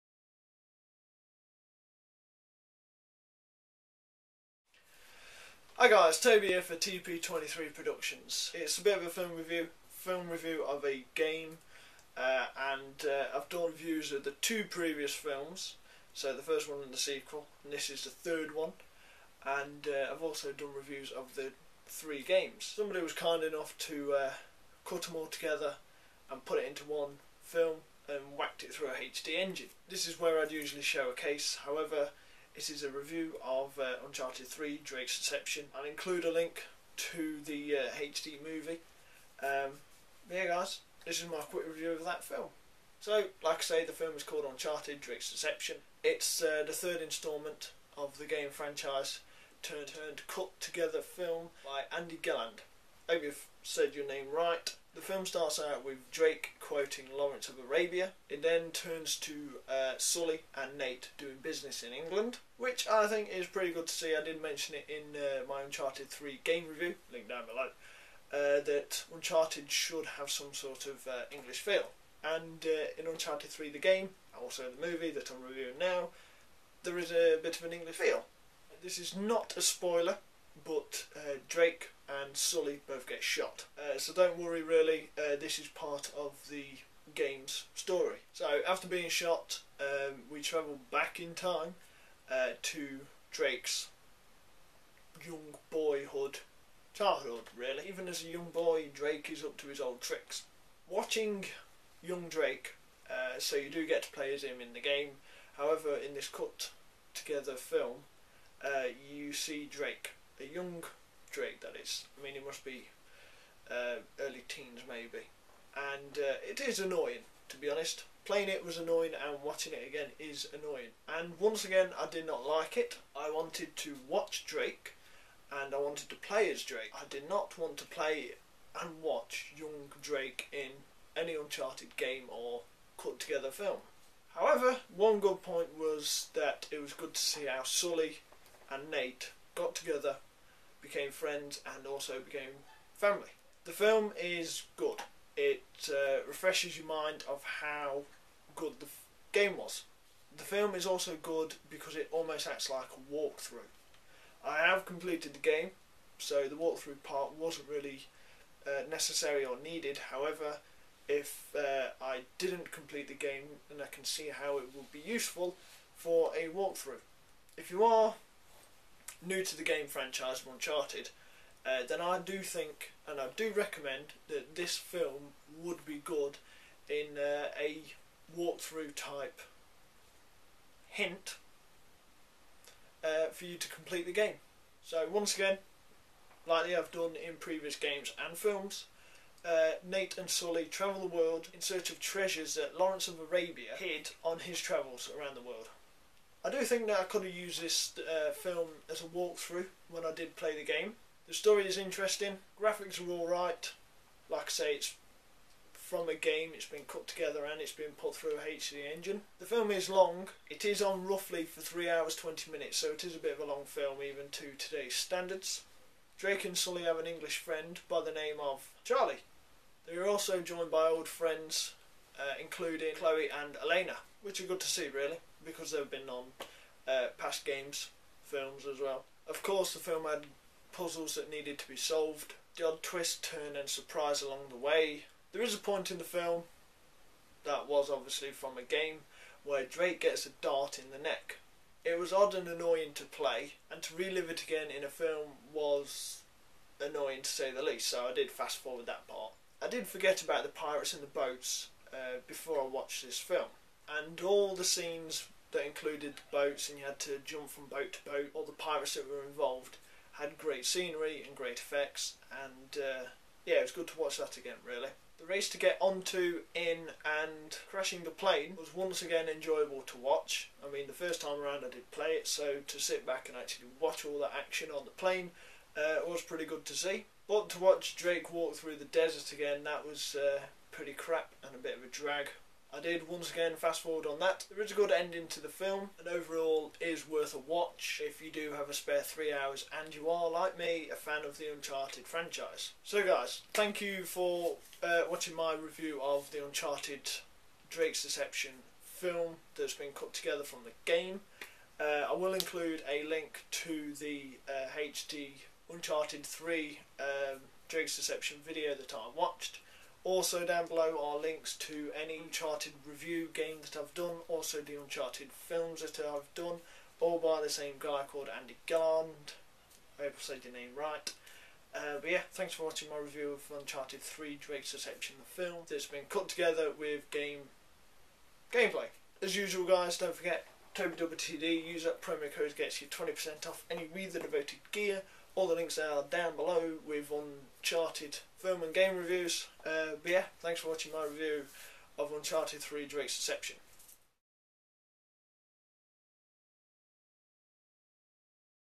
Uncharted 3 Drake's Deception Film Review
Hi Guys, Here Is My Review Of The Film, Uncharted 3: Drake's Deception.